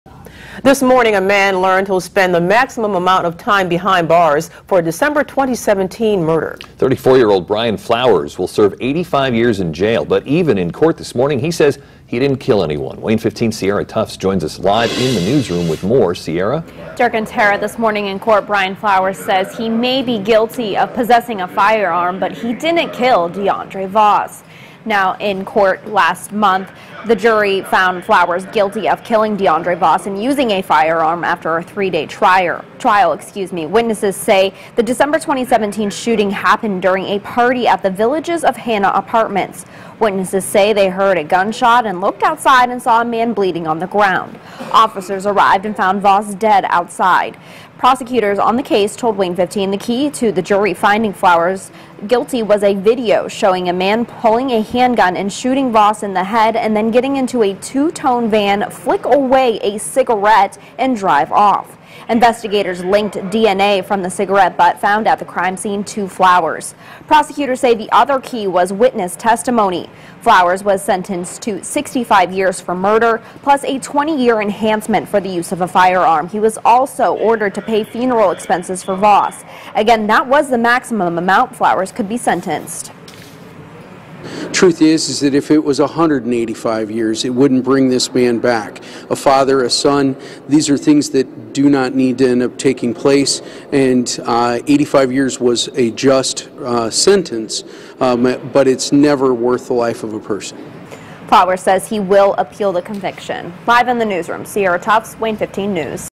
Man tied to killing through DNA sentenced
Bryan L. Flowers, 34, will serve 85 years for the Dec. 23, 2017, killing of De'andre Anthony Voss in the 1400 block of Greene Street.